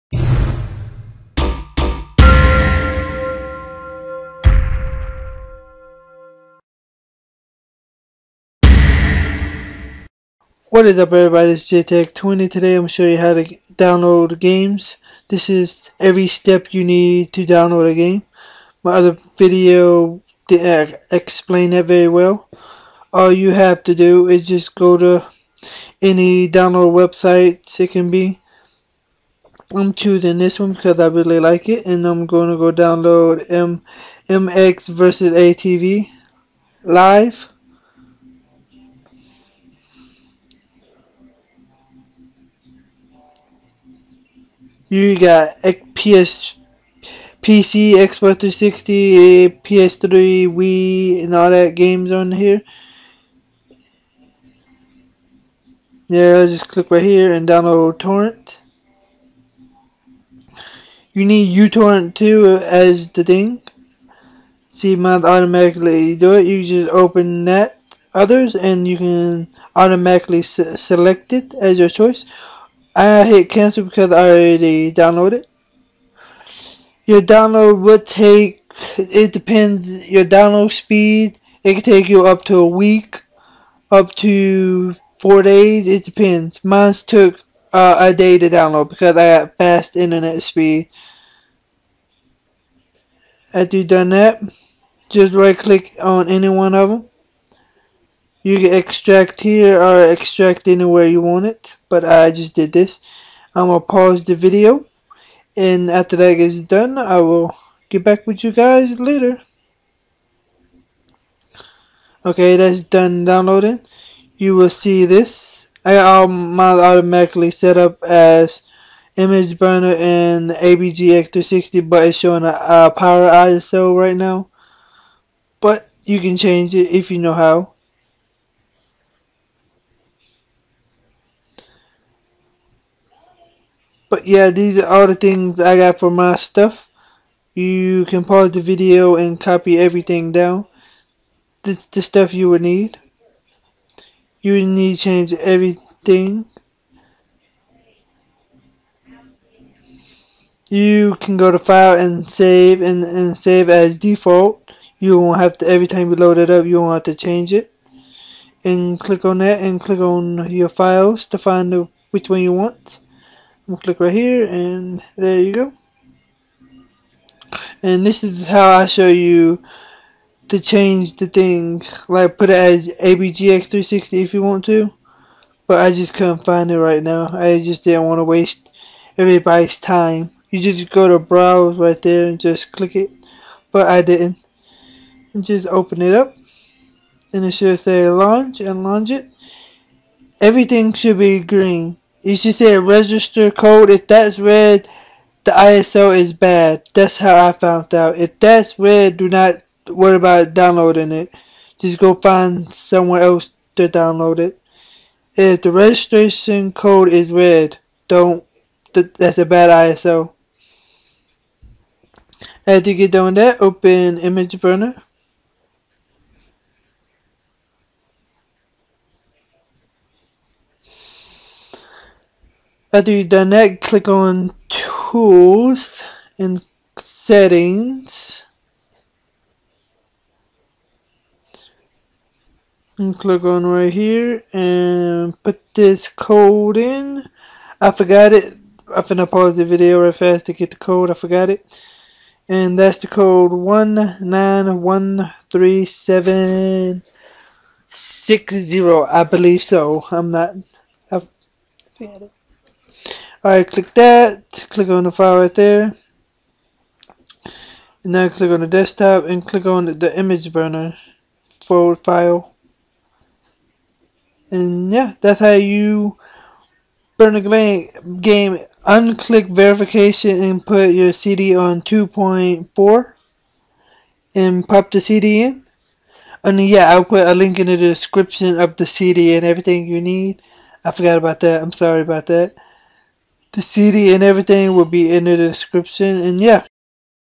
How to download game for xbox 360 *FULL TUT*
How to download game for xbox 360.

Imgburn
Sectors in L0 Code: 1913760
Here are the downloads to all the programs needed-

Disks You Need - amazon.co.uk/Verbatim-43666-Laye...QY/ref=...

Your Gonna Need To Flashed Xbox it Will Work !

30 Likes On This Video? Thanks :)

Credit For This Glitch - Unknown